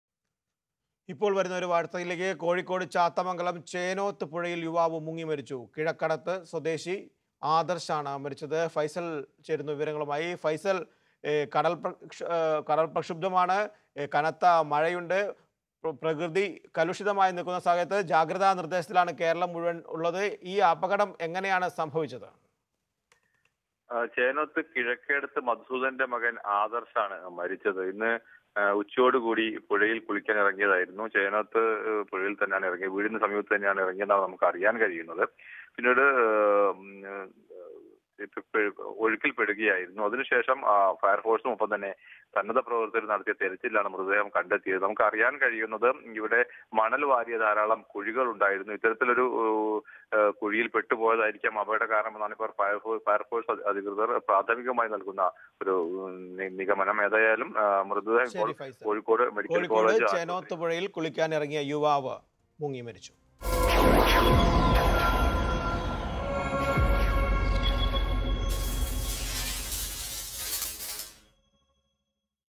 കോഴിക്കോട് 19കാരൻ പുഴയിൽ മുങ്ങിമരിച്ചു Youth drowned to death in Kozhkkode
കോഴിക്കോ‌‌ട് പുഴയിൽ കുളിക്കാനിറങ്ങിയ പത്തൊമ്പതുകാരൻ മുങ്ങിമരിച്ചു, ചാത്തമം​ഗലം ചേനോത്ത് പുഴയിലാണ് മുങ്ങി മരിച്ചത്
19 year old boy  drowned to death in Kozhkkode